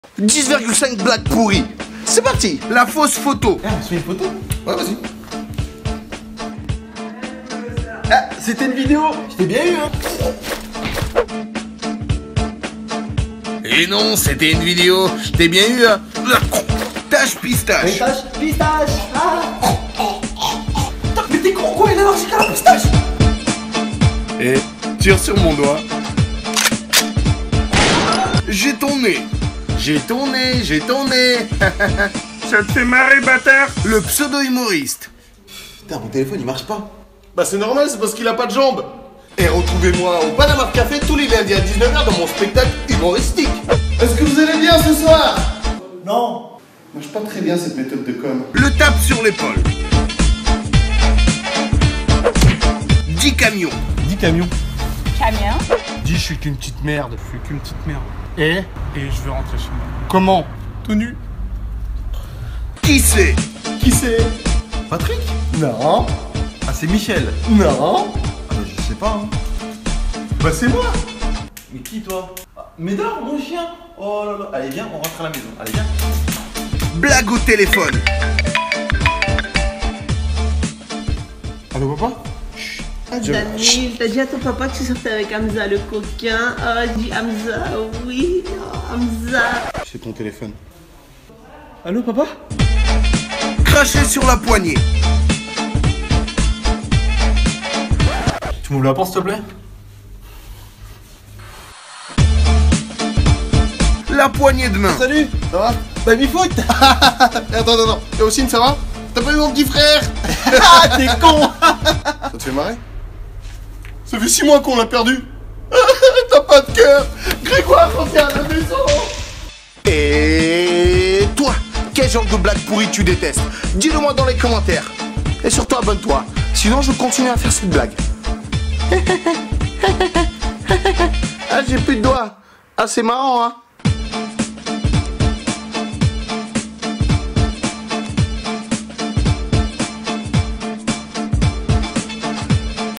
BLAGUES POURRIES - Daniil le Russe
Les pires blagues qu'on vous fait (ou que vous faites...)
Et vous, quelles blagues détestez vous?

Guest :
- Hocine Power
- Momo Bente
- 2Paks
- Lyes (NiataTV)
- Malaika

Liste blagues :

- Fausse photo
- Tache, pistache
- Tire sur mon doigt
- J'ai ton nez
- Pseudo Humoriste
- Tape sur l'épaule
- Dis camion
- Qui c'est ?
- Blague téléphone
- Cracher sur poignée
- La poignée de main